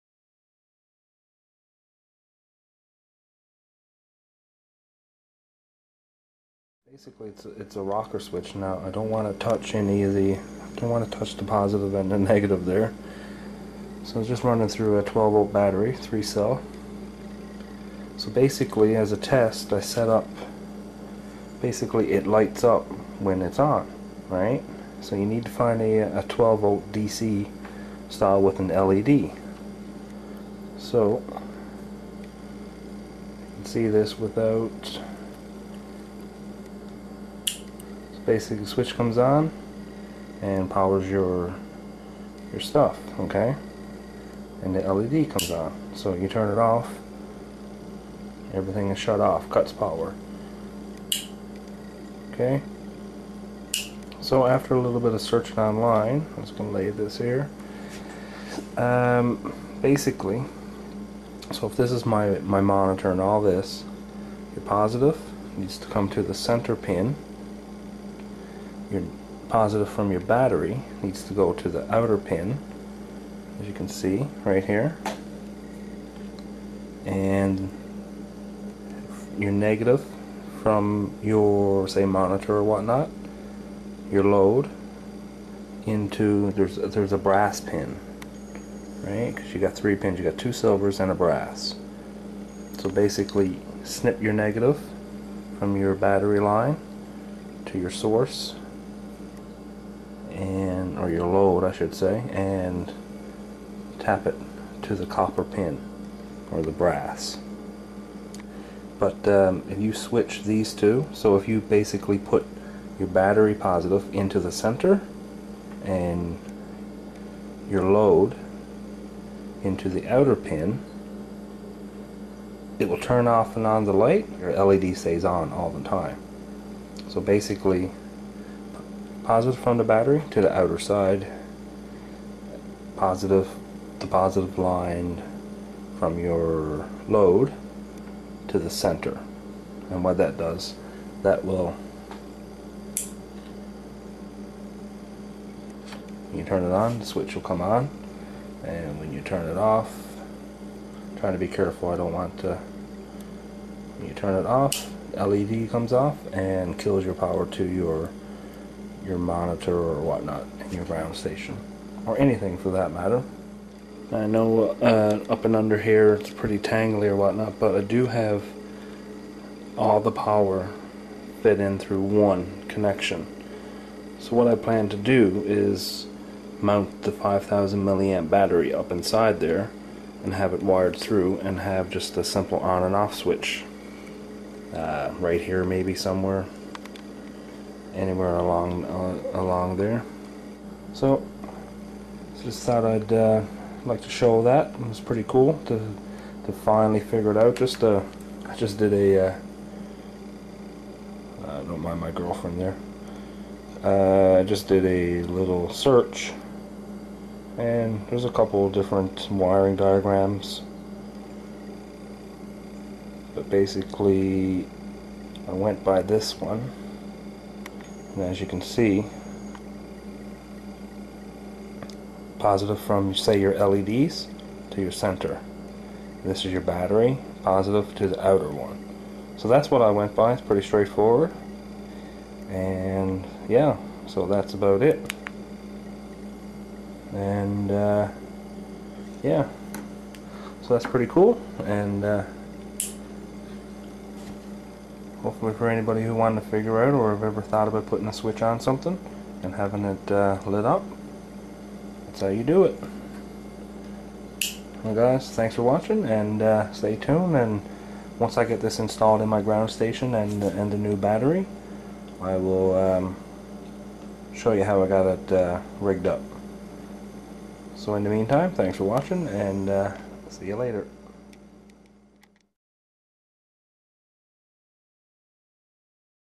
How to wire a LED Rocker Switch
This video is showing how to wire a Rocker toggle switch with a LED indication light.